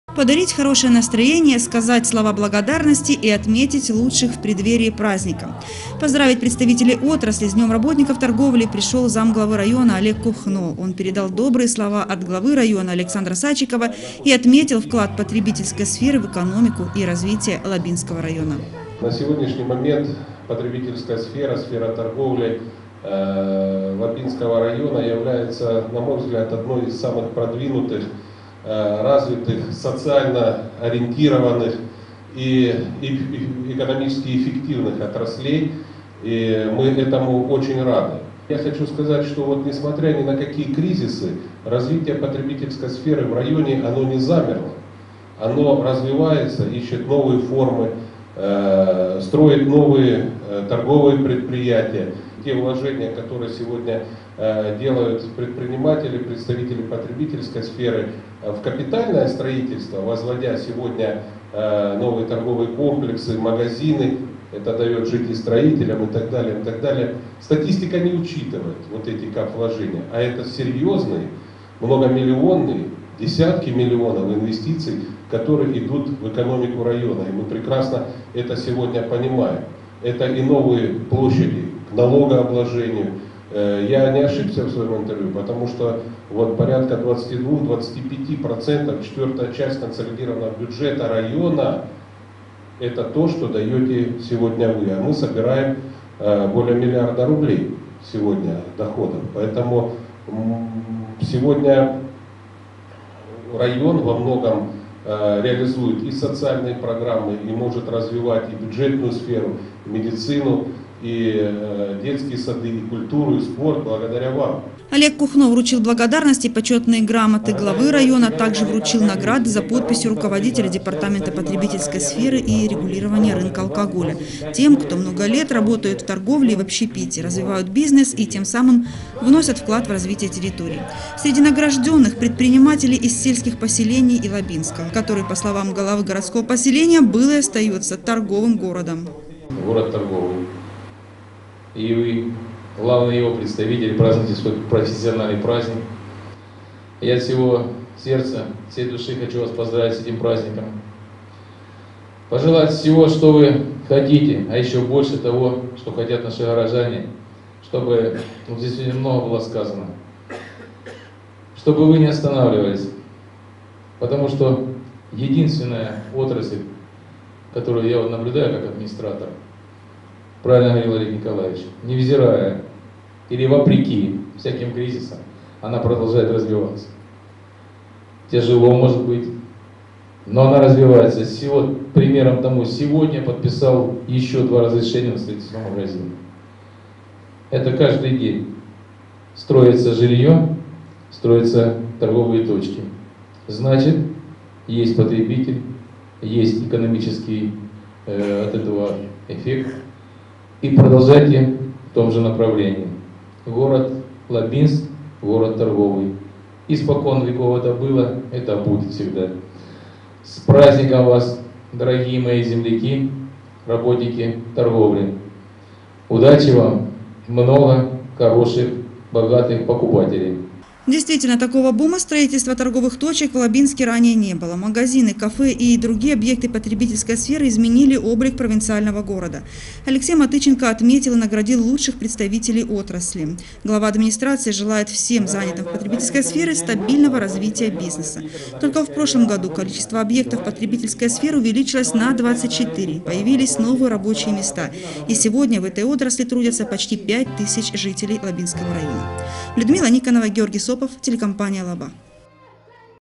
День работников торговли.
День работников торговли в этом году отмечается 23  июля.  В преддверии  праздничной даты в ГДК чествовали  представителей отрасли.